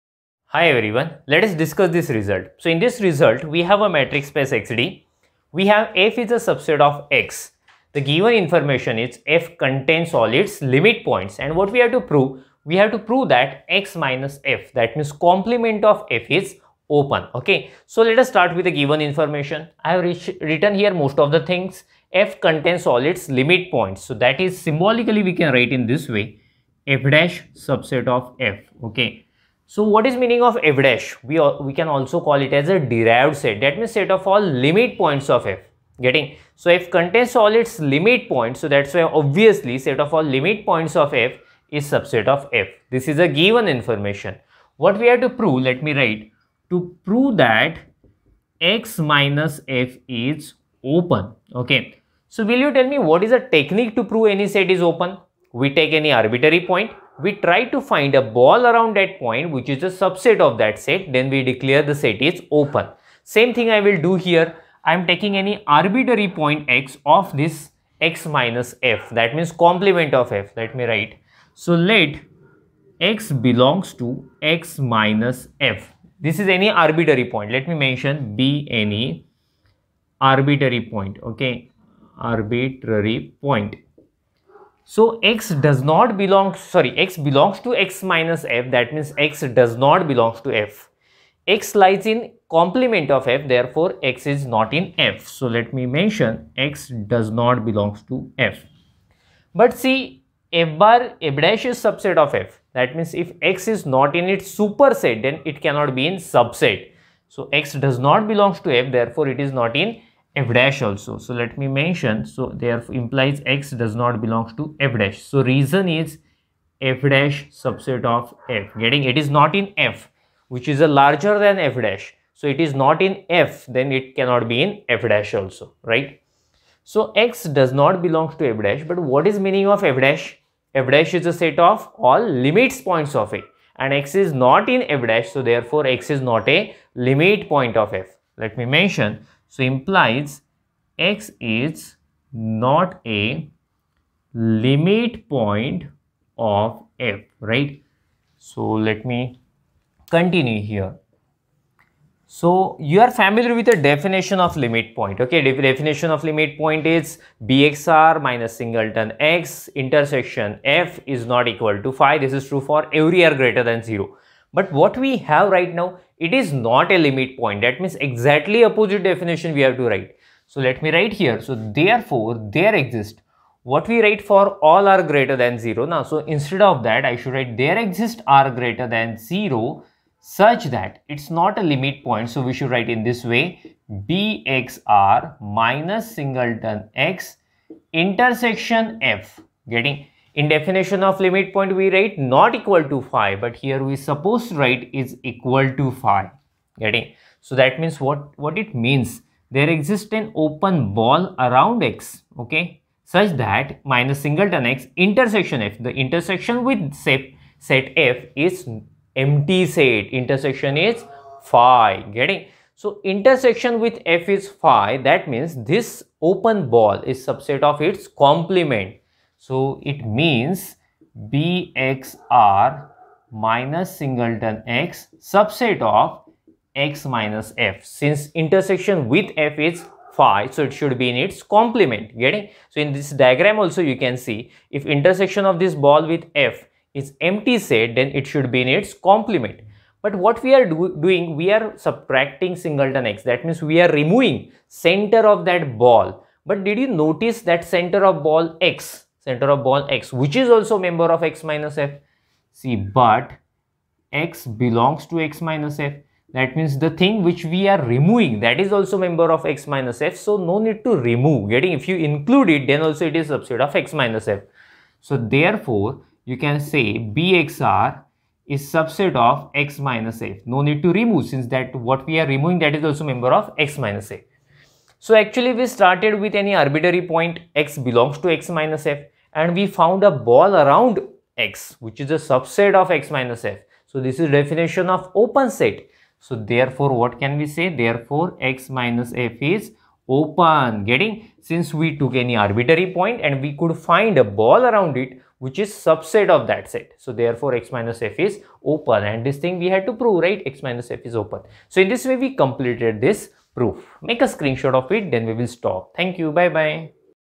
Metric Spaces | Lecture 50 | If a set contains all its limit points then its Complement is Open.
If a set contains all its limit points then its Complement is Open.